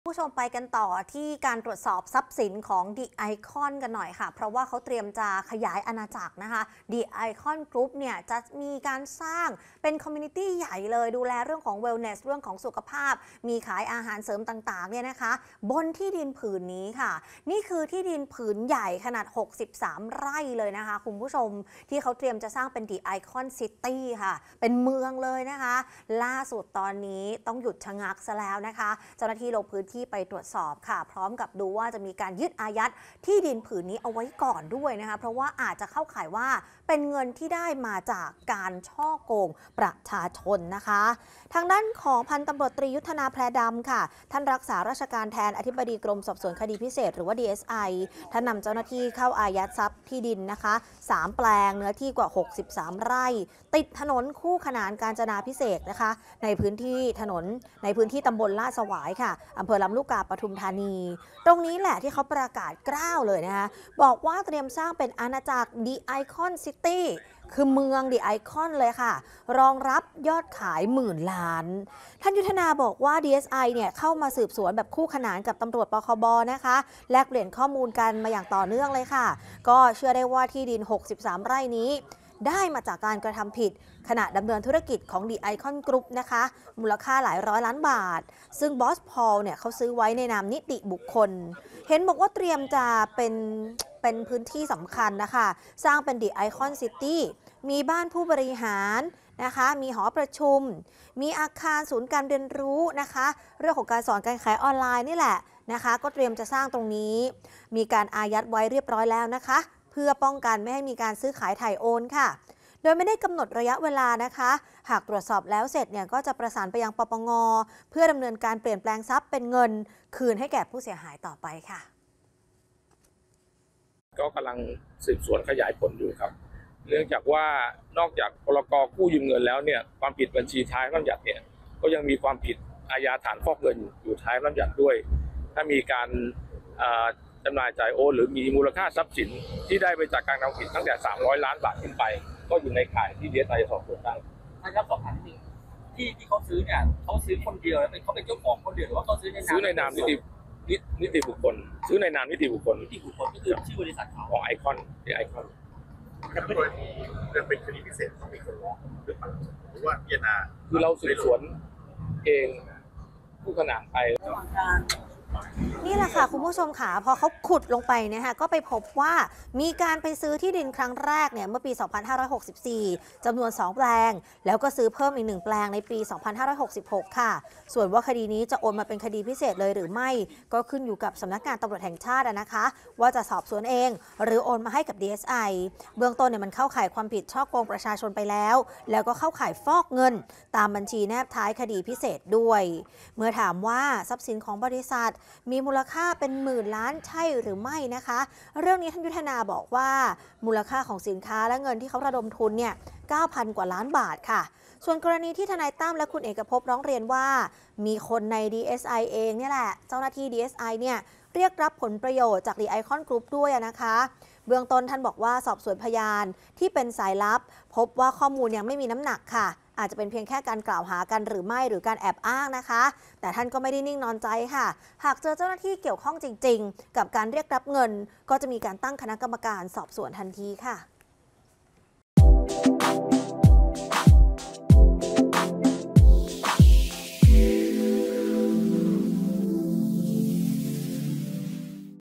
ดีเอสไอ ยึดที่ดิน “อาณาจักร ดิ ไอคอน ซิตี้” | โชว์ข่าวเช้านี้ | 17 ต.ค. 67
กรมสอบสวนคดีพิเศษหรือดีเอสไอ  นําเจ้าหน้าที่เข้าอายัดทรัพย์สินที่ดิน 3 แปลง เนื้อที่กว่า 63 ไร่ ติดถนนคู่ขนานกาญจนาภิเษก ในพื้นที่ ต.ลาดสวาย อ.ลำลูกกา จ.ปทุมธานี ที่เตรียมสร้างเป็น “อาณาจักร ดิ ไอคอน ซิตี้” รองรับยอดขายหมื่นล้าน